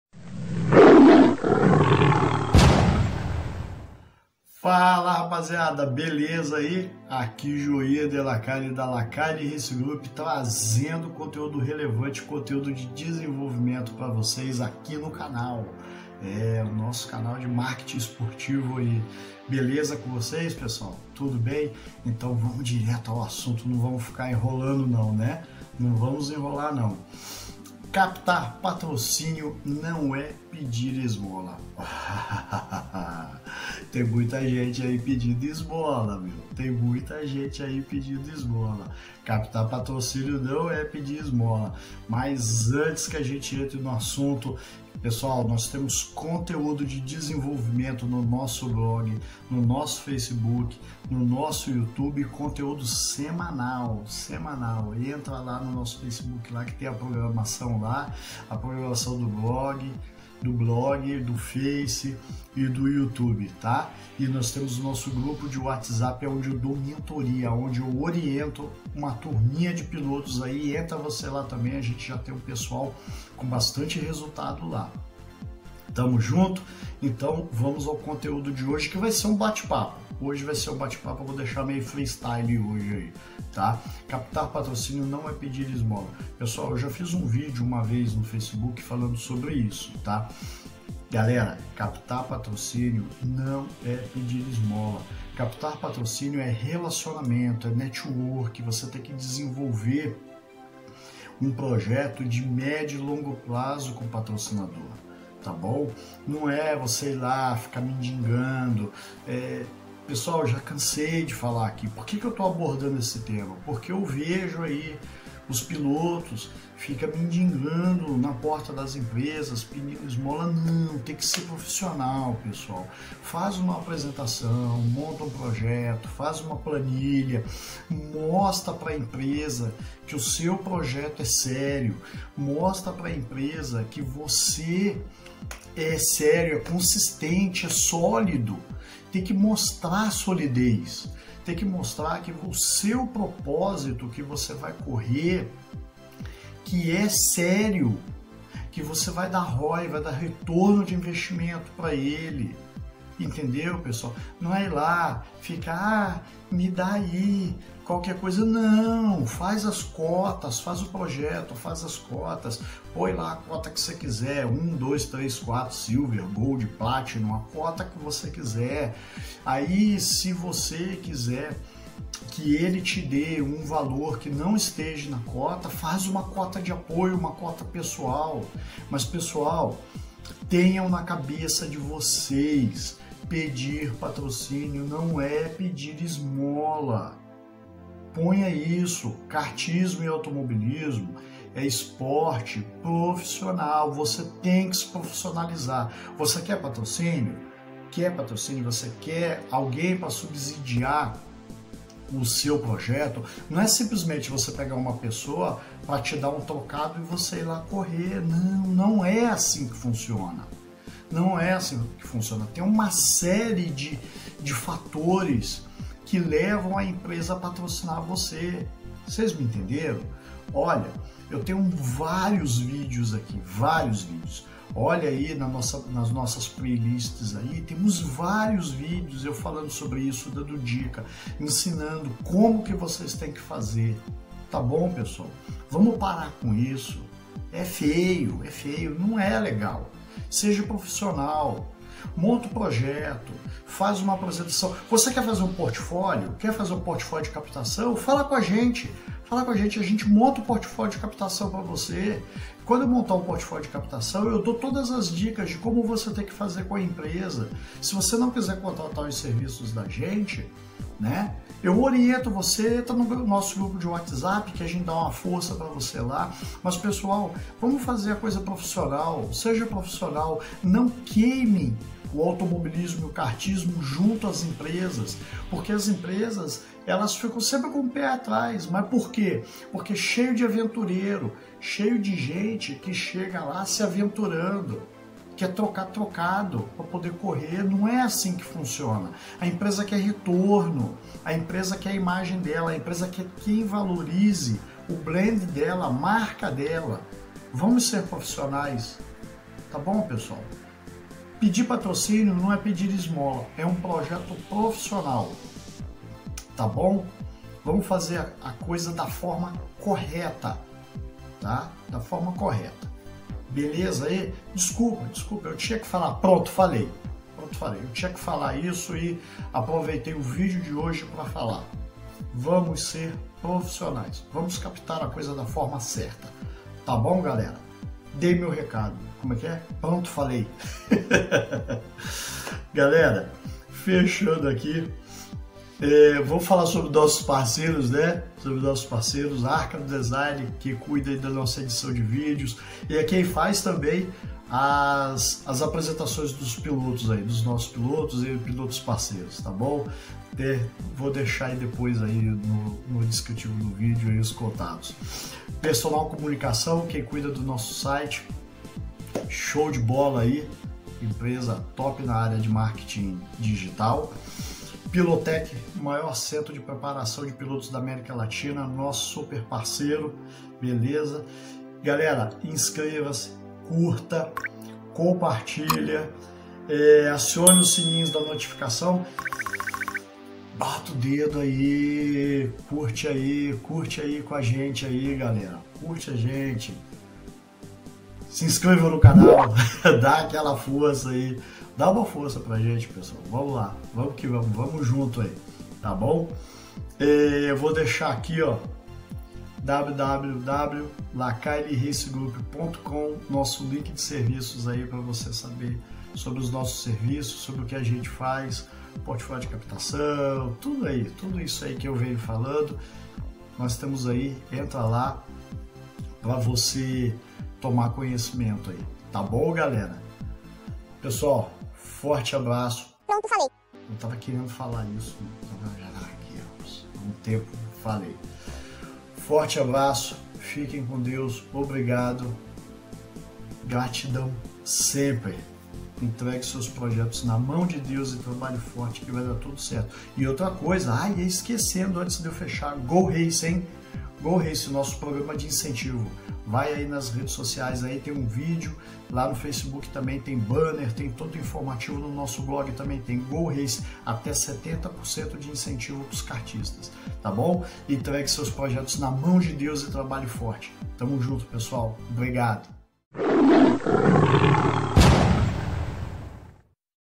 CAPTAR PATROCÍNIO NÃO É PEDIR ESMOLA
NÃO COMETA ESSE ERRO AO CAPTAR PATROCÍNIO ESPORTIVO, CAPTAR RECURSOS NÃO É PEDIR ESMOLA

Um dos maiores erros na captação de patrocínios esportivos e pedir esmola para as empresas, captação de patrocínio é relacionamento e via de mão dupla.

- Saiba mais ↴

PRODUÇÃO E EDIÇÃO DE VÍDEO: LRG - LACAILLE RACE GROUP